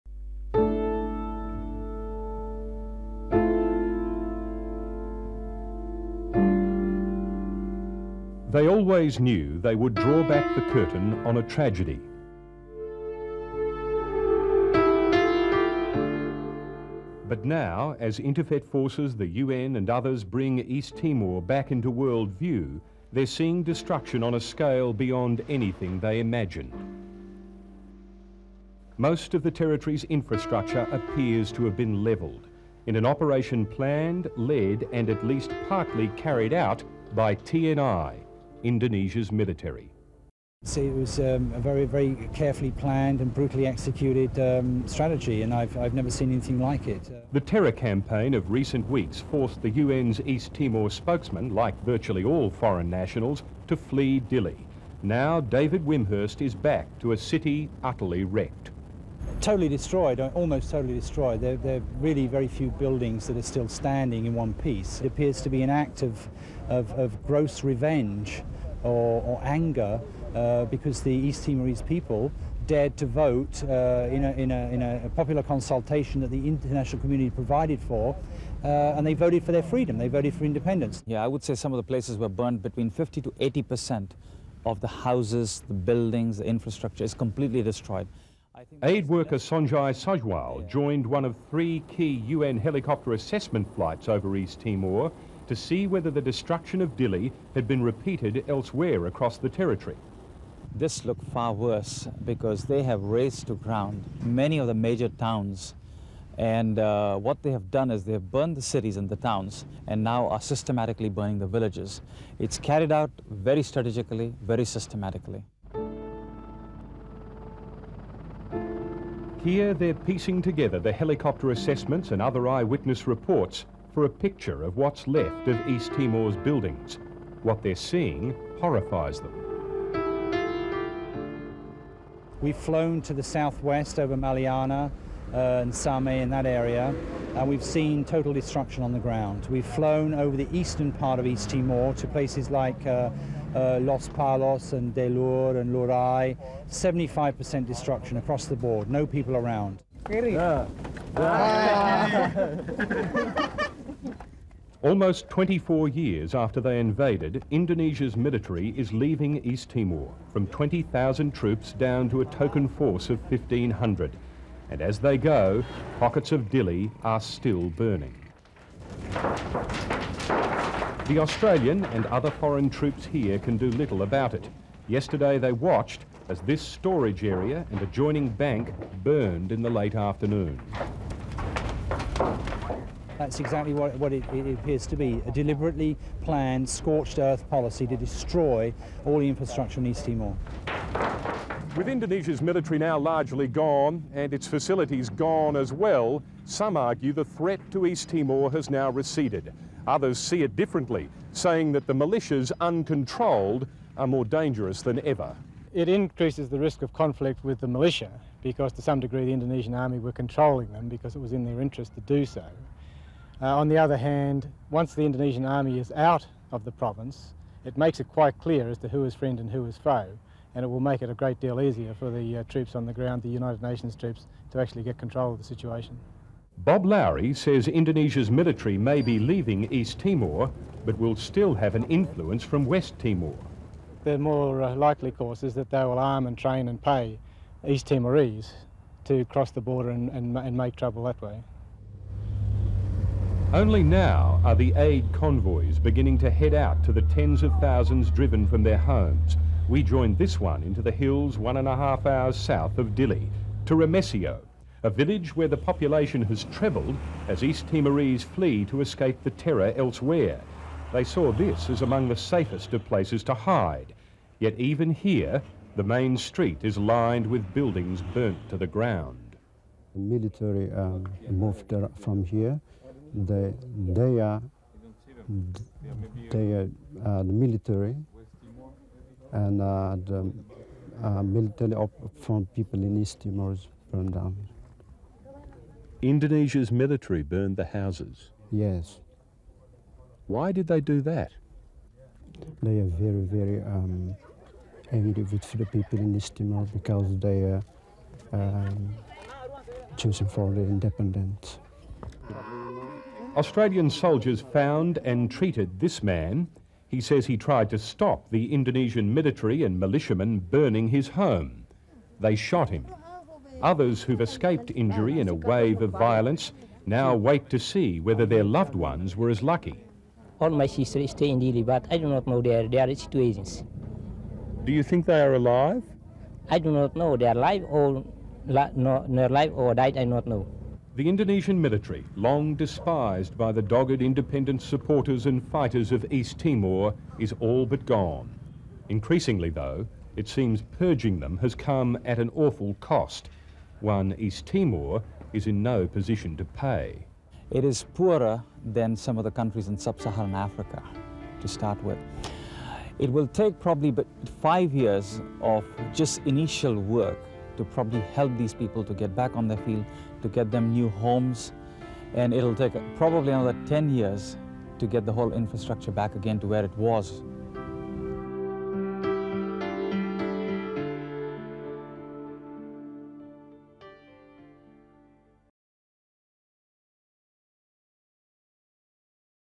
Footage from East Timor (1999)
Footage from East Timor following the aftermath of the historic 1999 elections.